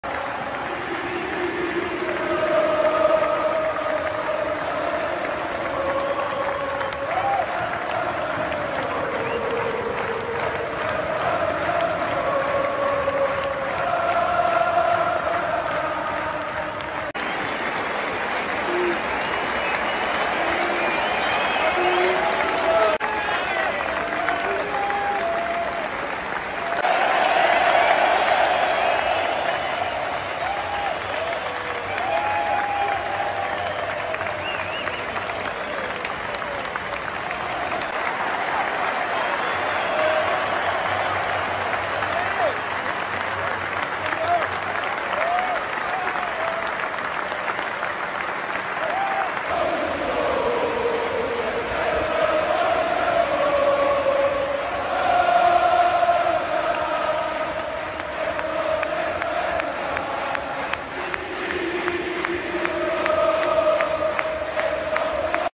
Goodbye! Roberto Firmino Emotional farewell to Liverpool fans
Heartbreaking scenes at Anfield as Roberto Firmino sheds tears while bidding farewell to his beloved Liverpool team. His supportive teammates comforted the Brazilian superstar during his emotional final match